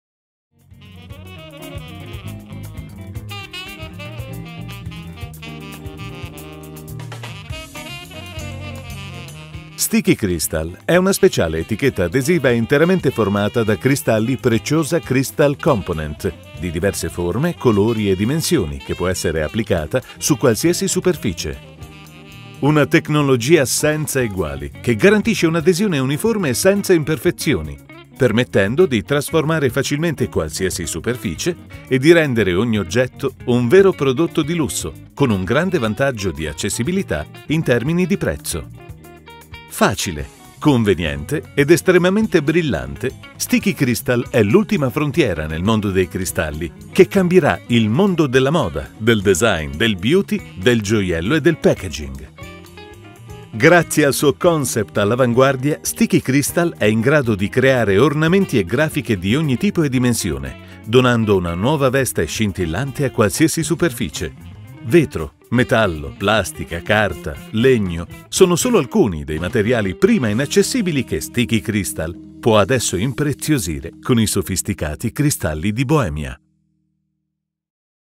Sticky Cristal 1 ITA
STICKY CRYSTAL™ è un innovativo adesivo con strass di diverse forme, colori e dimensioni che può essere applicato su qualsiasi superficie. Vetro, metallo, plastica, carta, legno: sono solo alcuni dei materiali che STICKY CRYSTAL™ può impreziosire con le sue tavolozze dalle cromie brillanti, aprendo a possibilità infinite laddove c’erano vincoli di applicazione.